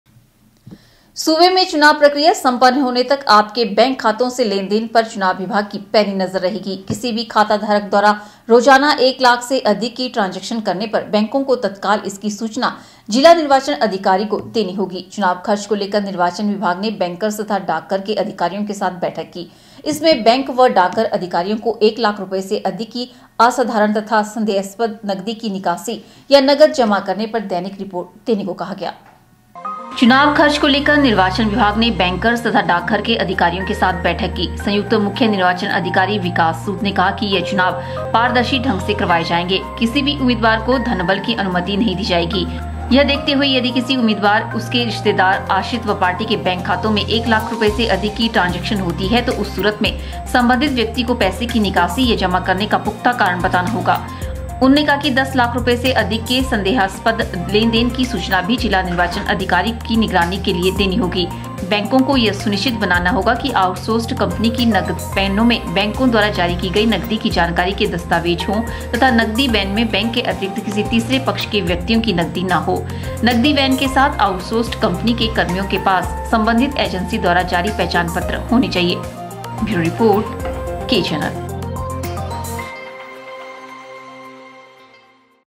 15.10.2017 फाइल: विधानसभा चुनाव रू बैंक खाते से निकासी व जमा पर रहेगी ई.सी. की नजर
फाइल: विधानसभा चुनाव रू बैंक खाते से निकासी व जमा पर रहेगी ई.सी. की नजर
10 लाख से अधिक के लेन-देन की देनी होगी सूचना